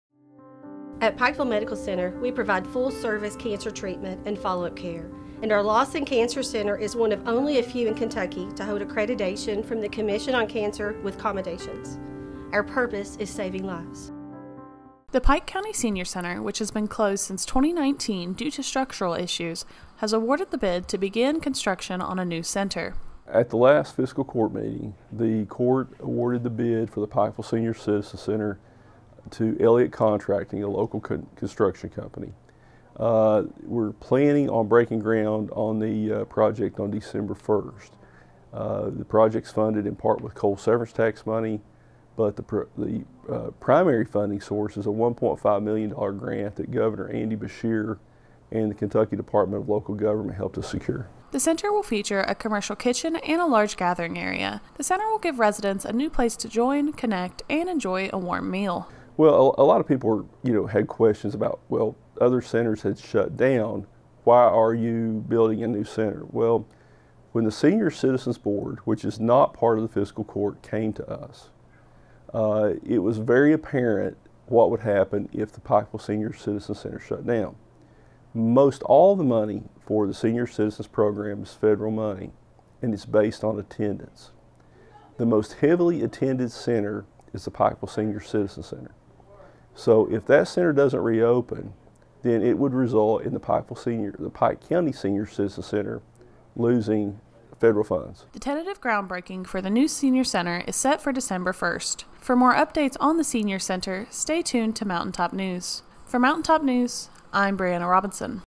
Bid Awarded to Pike County Senior Center
The Pike County Senior Center has been awarded a bid to begin reconstruction.